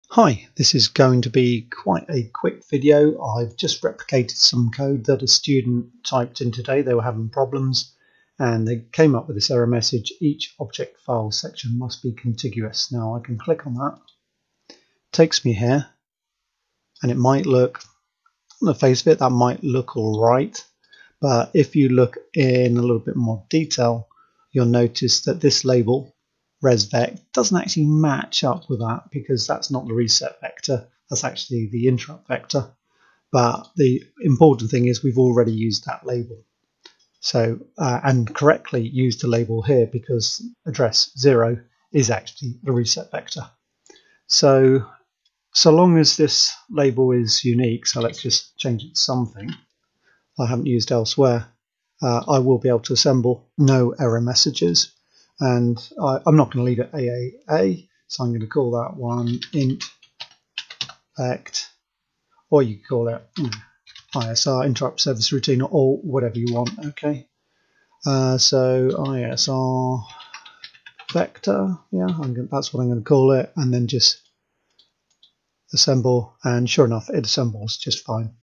Each object file section must be contiguous | MPASM assembler error message
One of my students had this error message in their code today. This is a quick video identifying the error and fixing the code.